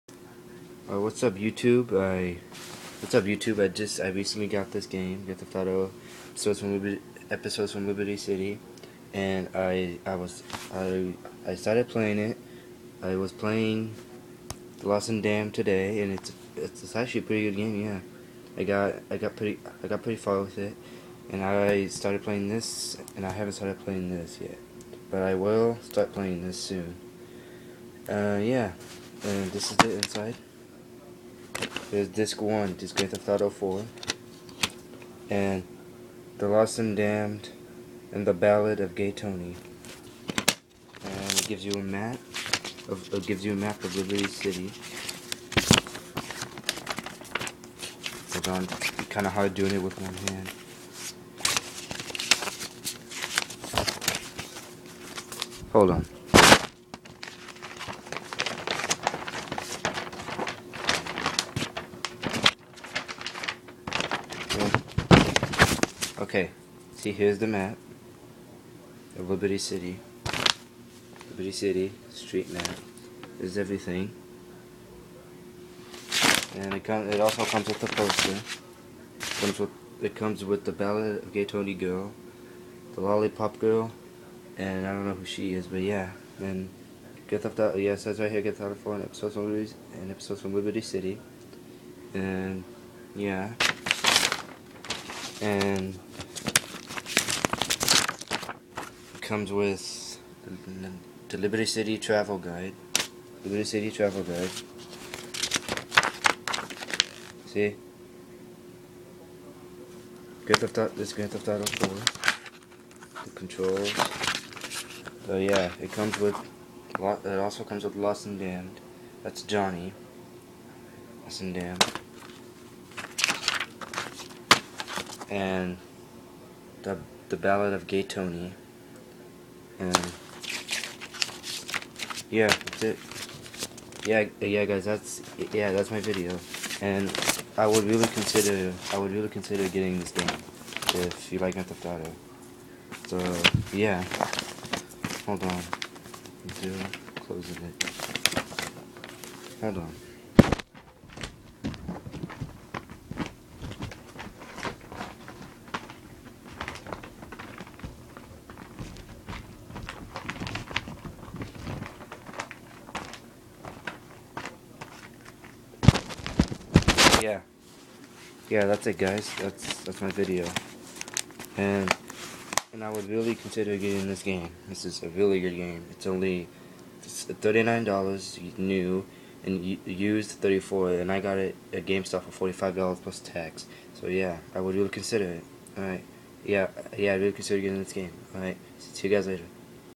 Grand theft auto 4. And episodes from liberty city unboxing
Me unboxing grand theft auto 4 and episodes from liberty city it's a really good game both of them lost and damned and balled of gay tony I would recommend getting this game and dont forget to comment rate and subscribe thnx.